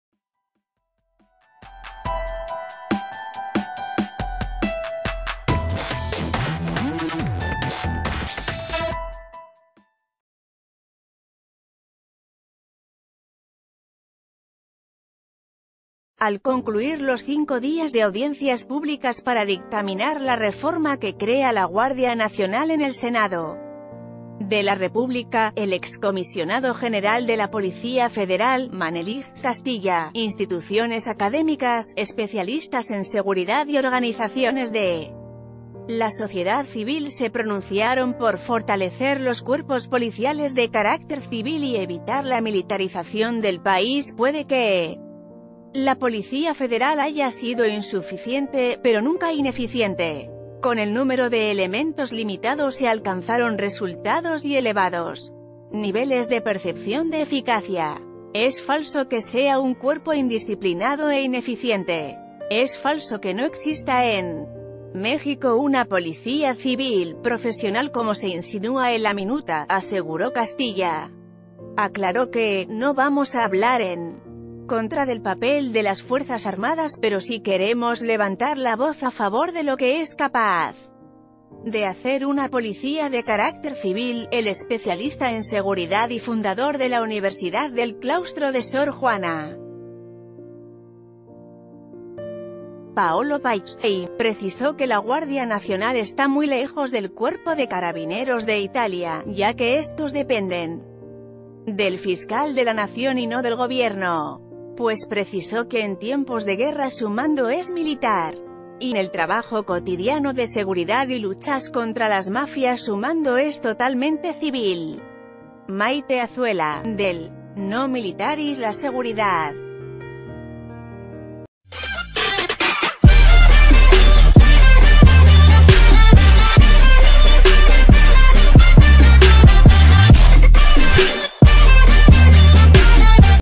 No fue ineficiente la Policía Federal, defiende Manelich Castilla
Al concluir los cinco días de audiencias públicas para dictaminar la reforma que crea la Guardia Nacional en el Senado de la República, el excomisionado General de la Policía Federal, Manelich Castilla, instituciones académicas, especialistas en seguridad y organizaciones de la sociedad civil se pro...